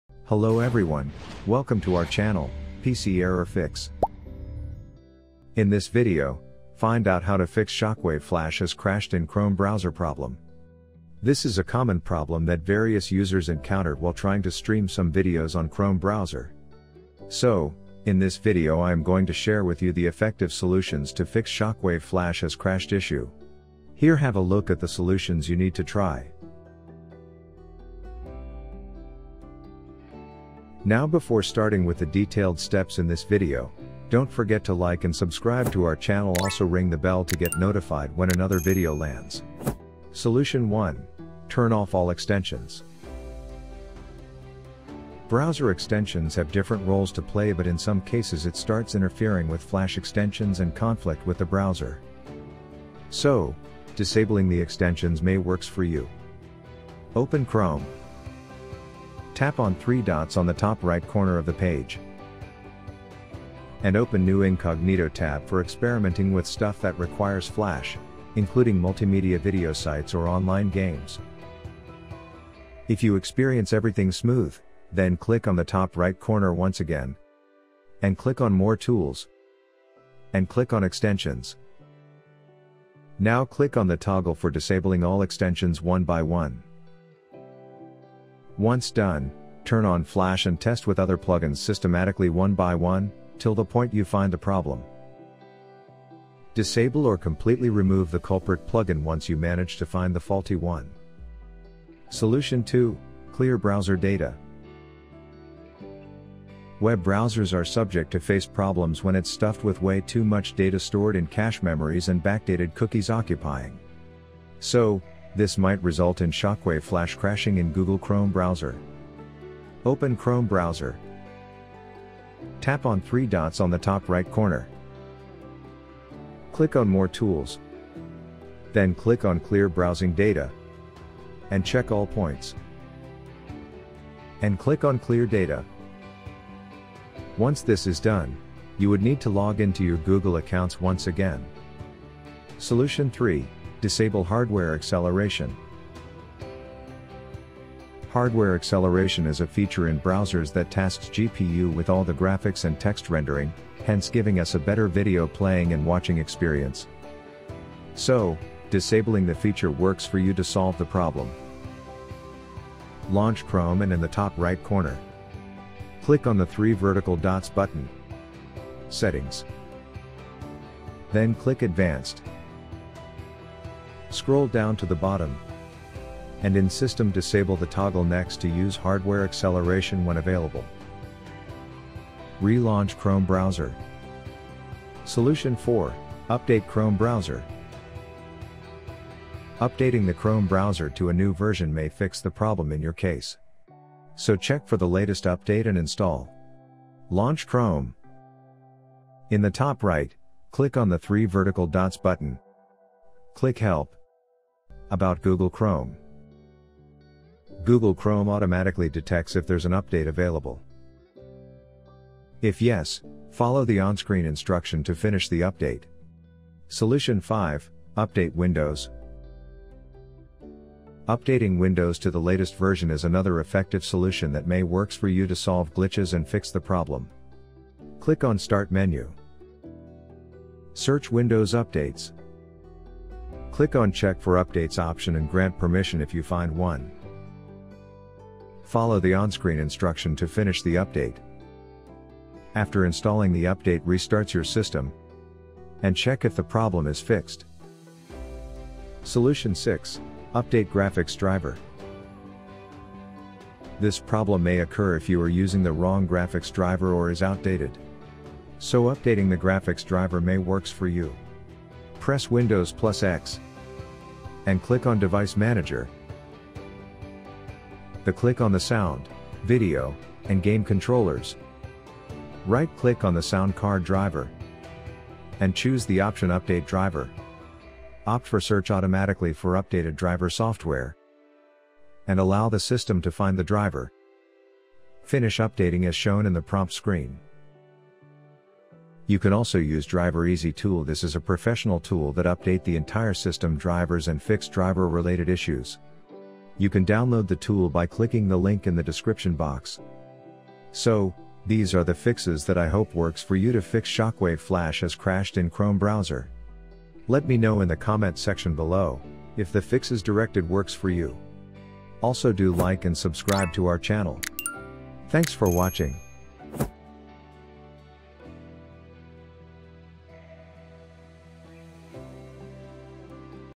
Shockwave Flash has Crashed in Google Chrome | Working Tutorial | PC Error Fix
While streaming some videos on Chrome seeing the error  message Shockwave Flash has crashed in Google Chrome then watch the video till the end and follow the fixes shown.

Key Moments:

0:00 Intro
0:40 Solution 1: Turn off All Extensions
1:42 Solution 2: Clear Browser Data
2:24 Solution 3: Disable hardware acceleration
3:06 Solution 4: Update Chrome Browser
3:38 Solution 5: Update Windows
4:10 Solution 6: Update graphics driver